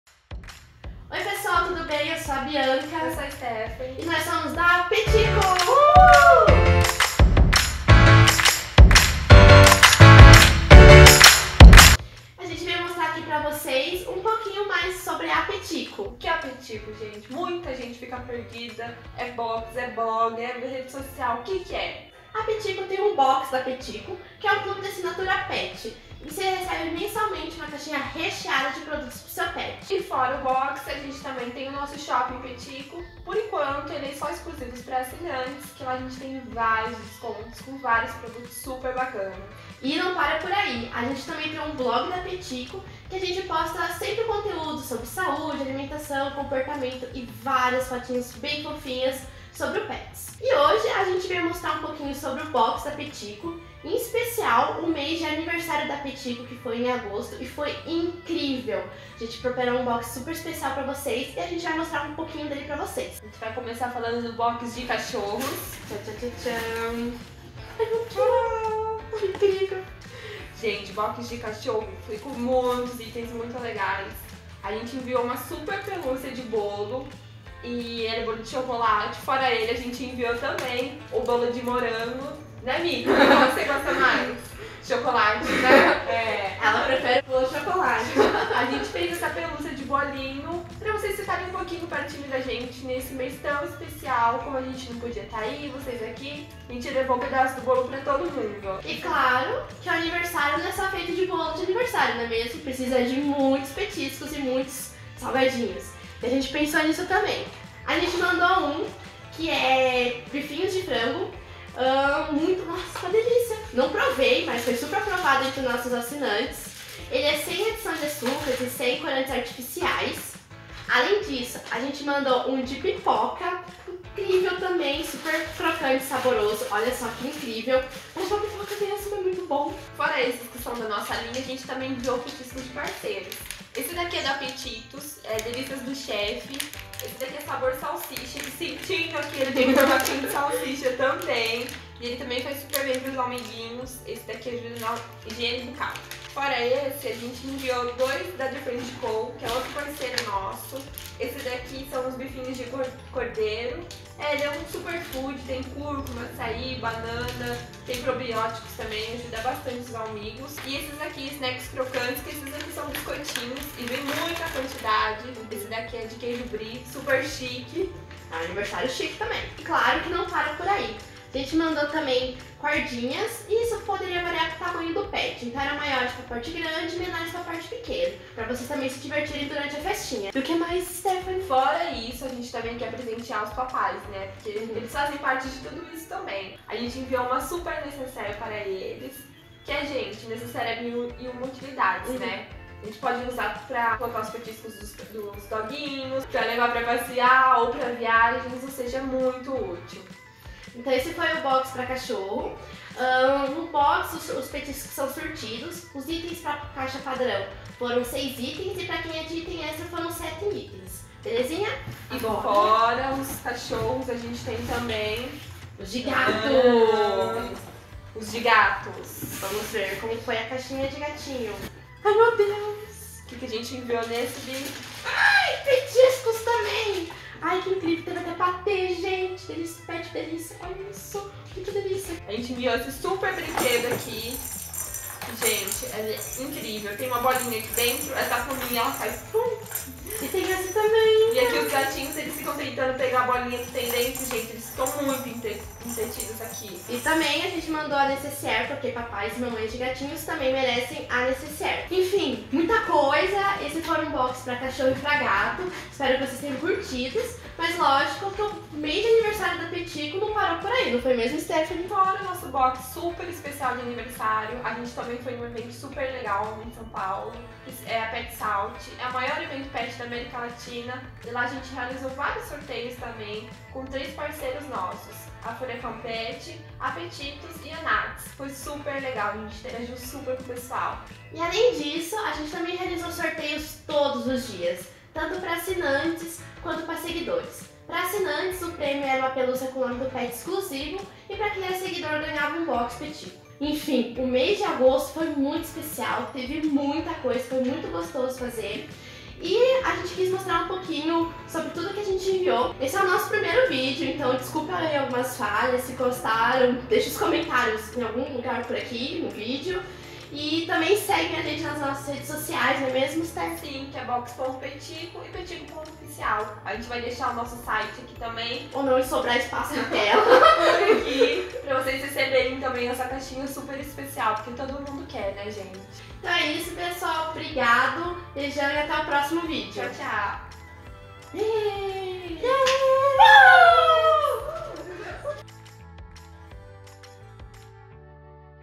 ABRINDO BOX.PETIKO AUNIVERSÁRIO - UNBOXING AGOSTO/2018
O mês de agosto foi muito especial porque a Petiko comemorou 3 anos!!! E levamos um pedaço da festa para cada um de nossos AUssinantes.

Confira o unboxing desta edição repleta de alegria e comemoração!

💻 Tem dúvidas sobre pets? Conheça nosso BLOG.Petiko e use a busca para encontrar o assunto